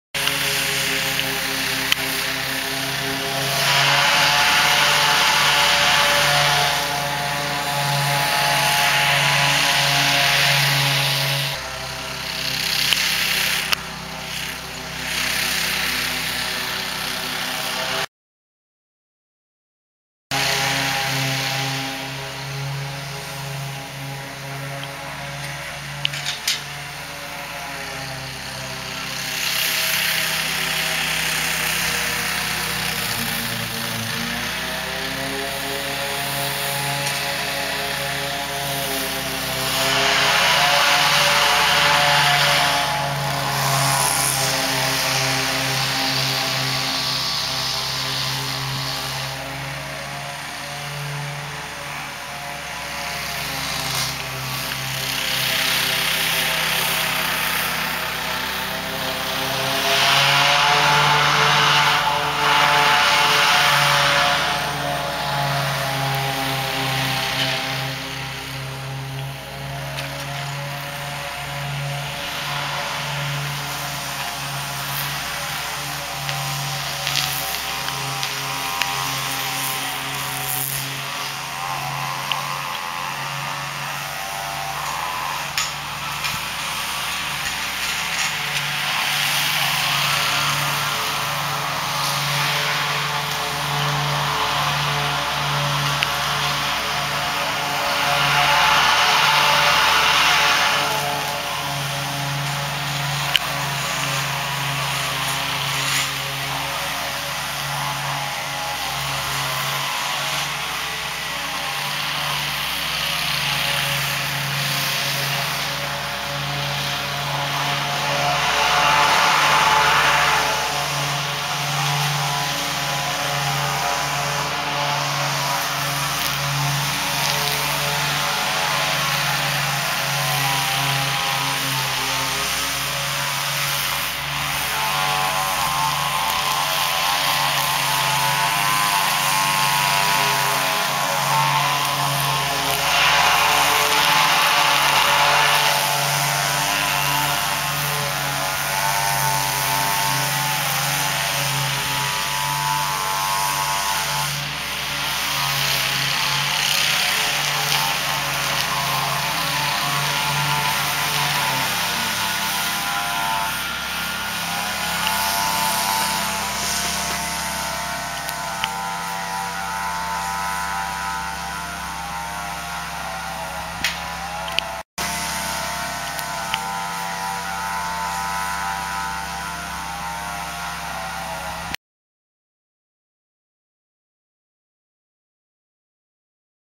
Great Dane Standup Mower for Lawnboy1010
Thanks for all the great AAA Trash footage - including that former AW truck in AAA colors.  Got you (and anyone else that's interested in mowers) a decent amount of footage of a local landscaper.

I think this is a Great Dane stand up mower.  Enjoy.